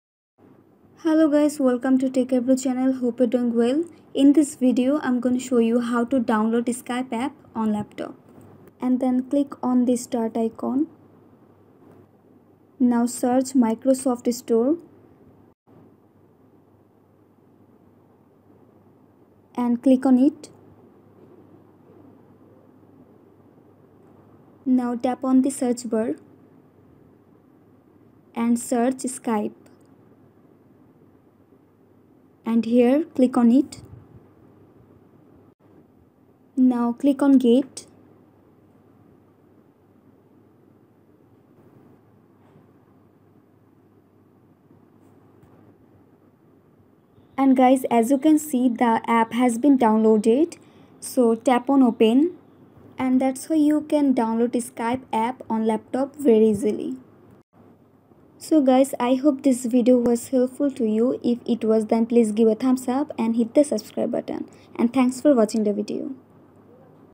How to Download Skype on Laptop 2025 [New Method] Step-by-Step Tutorial
Are you looking to download Skype on your laptop but don't know where to start? In this tutorial, we'll walk you through the steps to download Skype on your laptop, whether you're using Windows or Mac. We'll cover everything from finding the right download link to installing the software and creating an account. By the end of this video, you'll be able to connect with friends and family on Skype with ease.